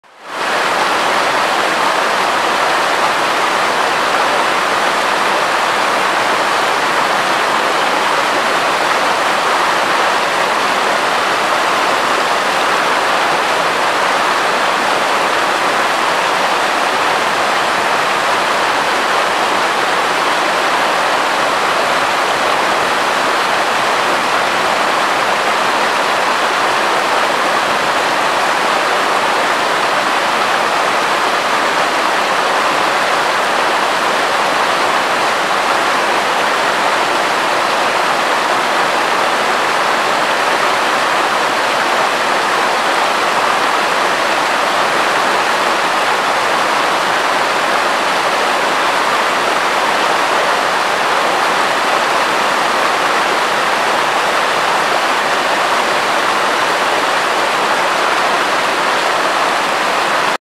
2016 Smoky Mountian National Park Apr 16 521
Chimneys Picnic Area. The Little Pigeon river coming down for the Chimney Tops in the Great Smokey Mountain National Park . My Favorite place in the world.